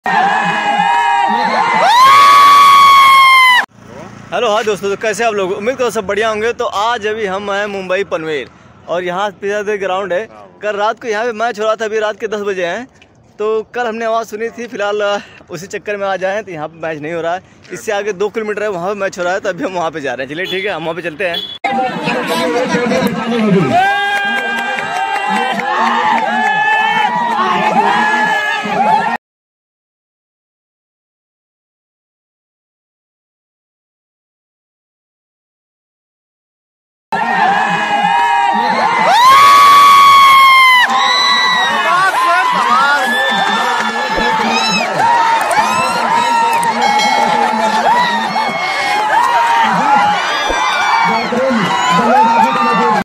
अंडरआर्म क्रिकेट में लास्ट बॉल पर चौका मार कर सेमीफाइनल में जगह बना लिया। underarm cricket.
Hello Friends,

अंडरआर्म क्रिकेट सेमीफाइनल
अंडरआर्म मैच
हाफ बाउंड्री मैच
Underarm turnament
लास्ट बॉल पर चौका मार